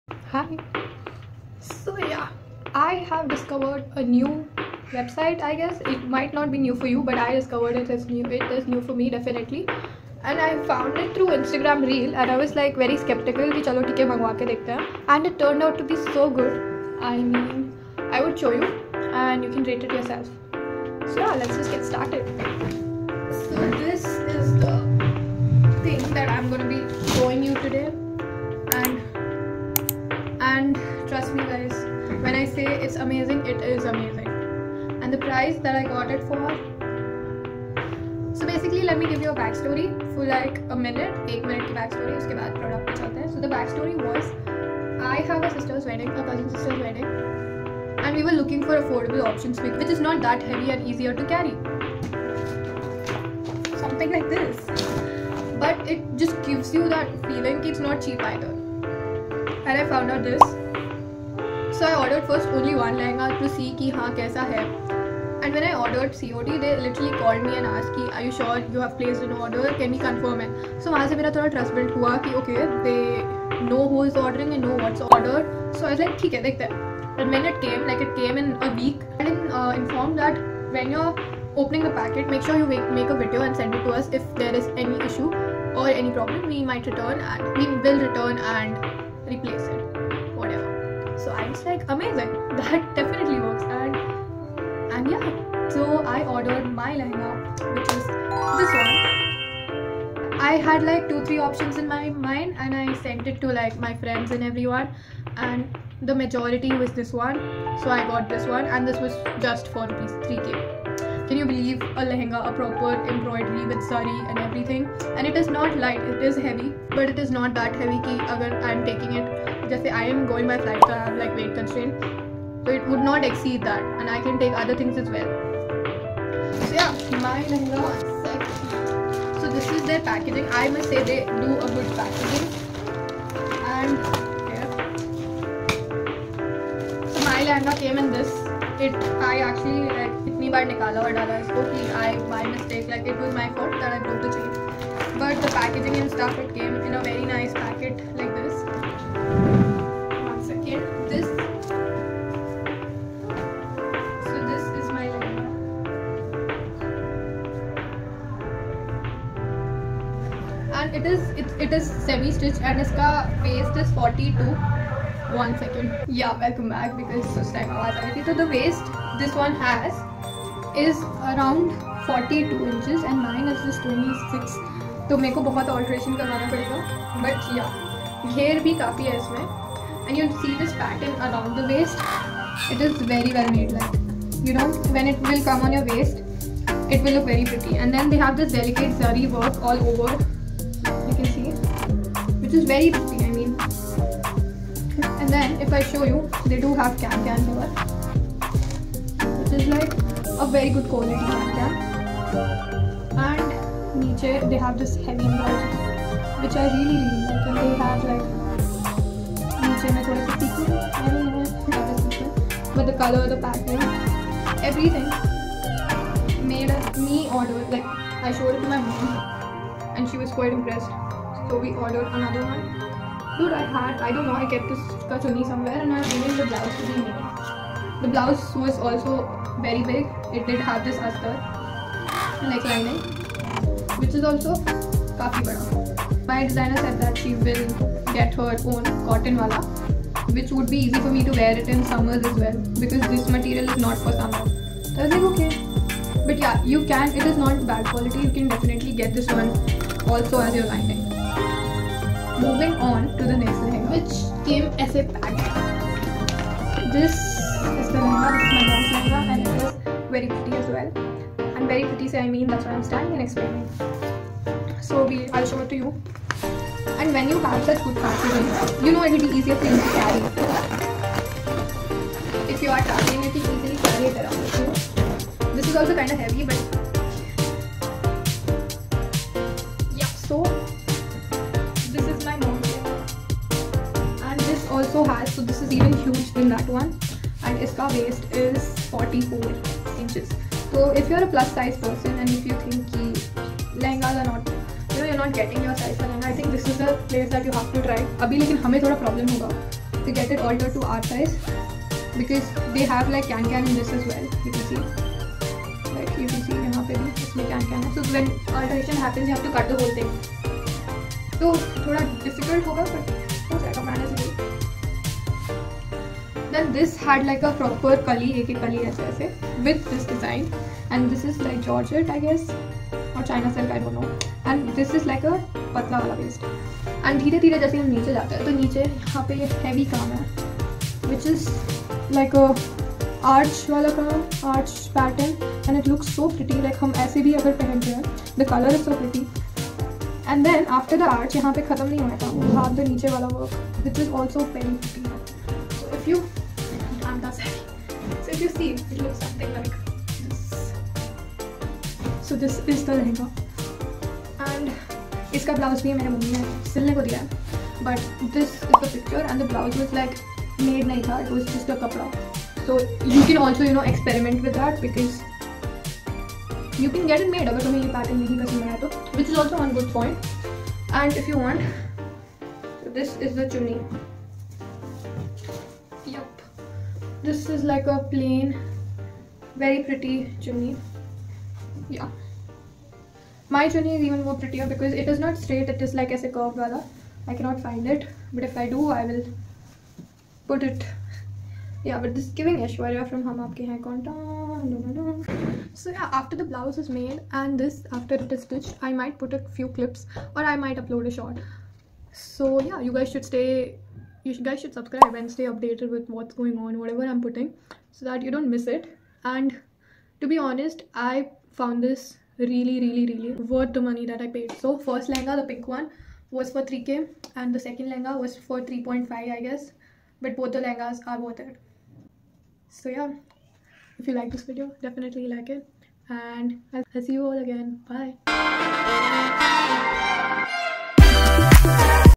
Designer Lehenga Website BL Fabrics Review | Designer Lehenga Under 4k 😱 | Aparna Dwivedi haul
Hey Shawty's
As the wedding season is back so am I with amazing lehngas